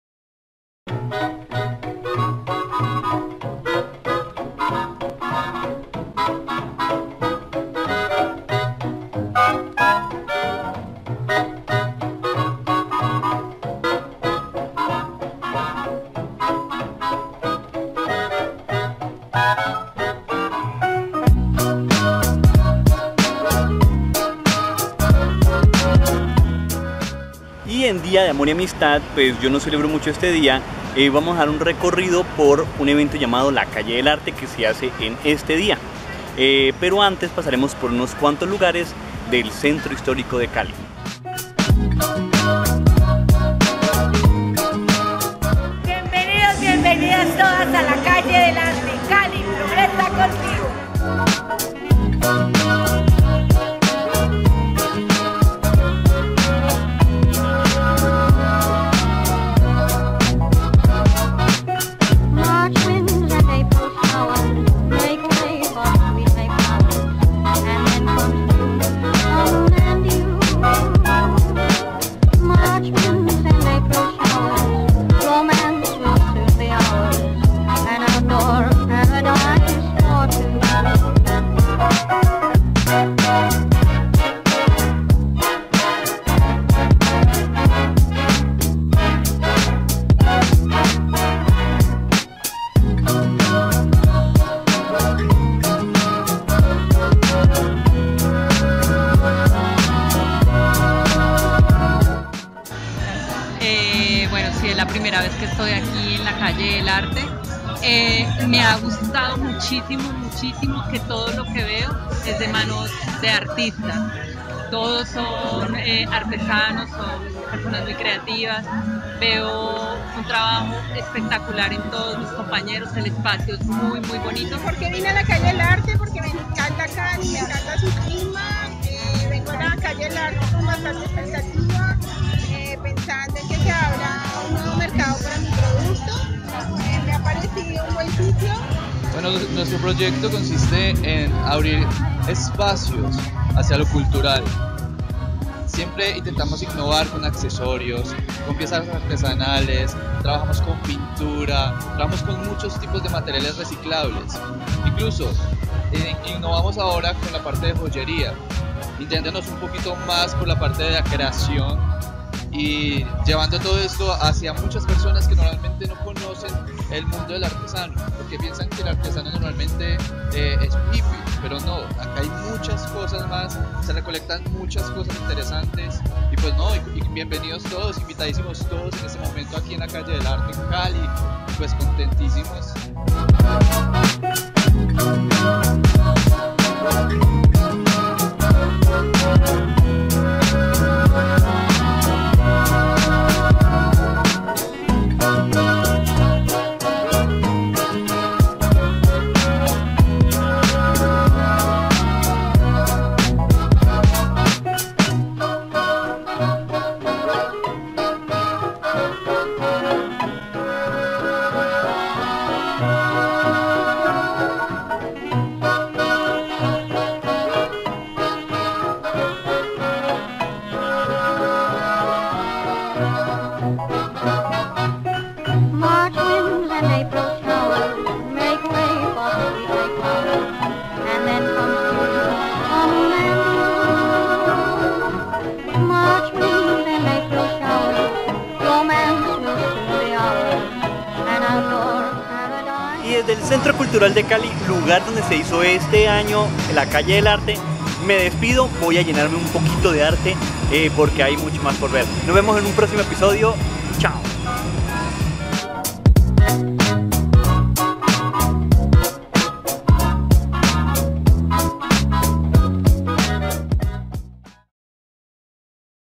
5 Recorriendo la calle del arte
En el día de amor y amistad decidimos pasar un rato por la calle del arte. Acompáñanos.